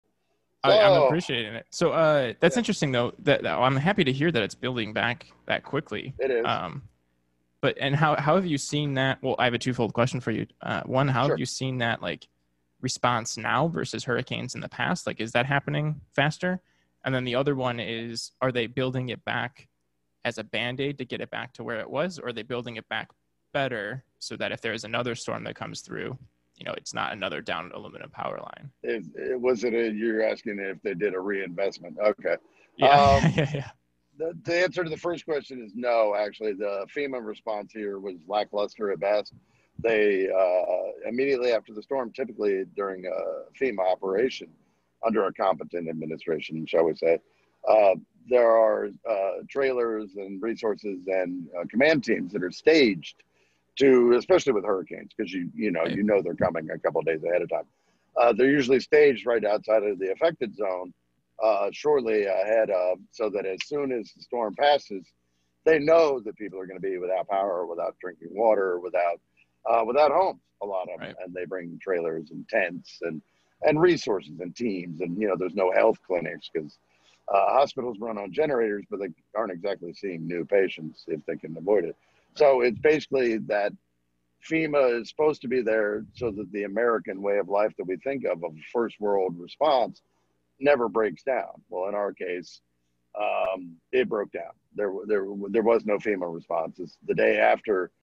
"how are they building back" - 003_ interview: let rob be rob
This is an excerpt from my conversation with with Rob Anderson, Congressional Candidate for Louisiana's 3rd district, about his uniquely American story, why he entered politics and the effects from hurricane Laura.